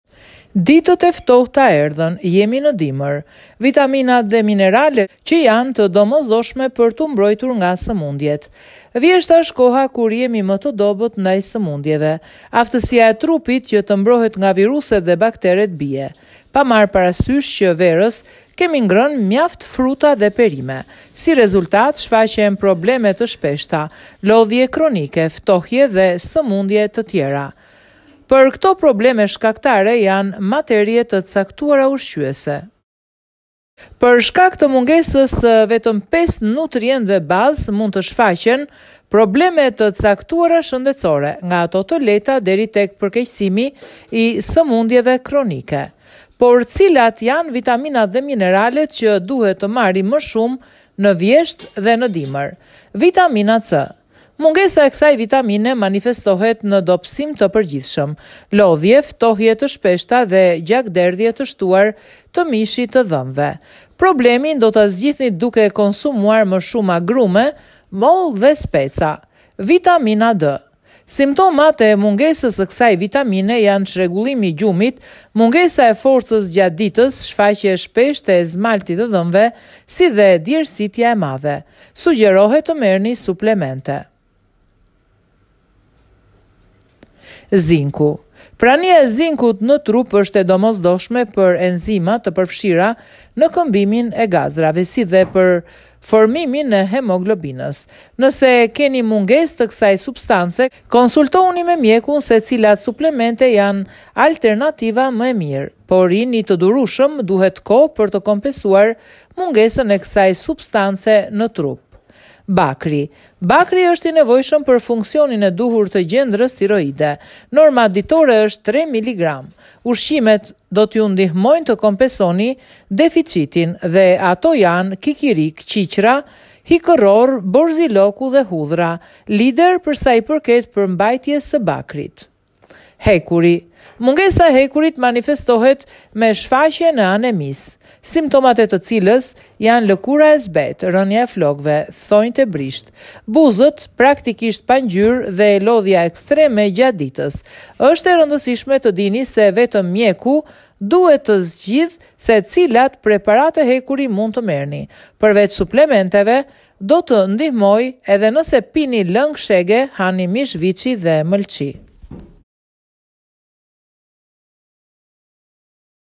Ditët e ftohta erdhën, vitaminat dhe mineralet që janë të domosdoshme për tu mbrojtur nga sëmundjet!
Vjeshta është koha kur jemi më të dobët ndaj sëmundjeve, aftësia e trupit që të mbrohet nga viruset dhe bakteret bie, pa marrë parasysh që verës kemi ngrënë mjaft fruta dhe perime. Si rezultat, shfaqen probleme të shpeshta, lodhje kronike, ftohje dhe sëmundje të tjera.